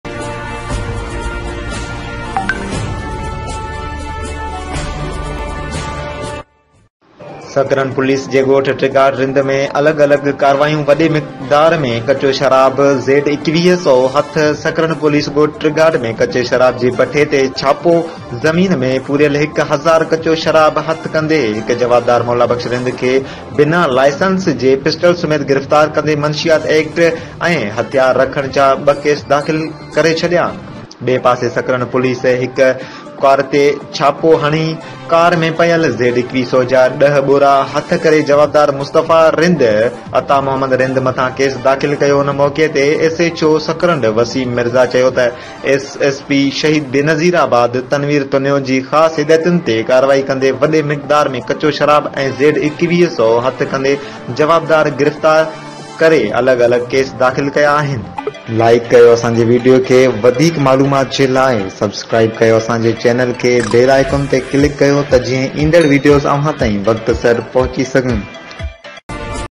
سڪرنڊ پوليس جي ڳوٽ ٽھگاٽ رند ۾ ٻه الڳ الڳ ڪاروايون
سڪرنڊ پوليس جي ڳوٽ ٽھگاٽ رند ۾ ٻه الڳ الڳ ڪاروايون وڏي مقدار ۾ ڪچوشراب ۽ زيڊ 2100سئو ھٿ سڪرنڊ پوليس ڳوٺ ٽھگاٽ ۾ ڪچي شراب جي بٺي تي ڇاپو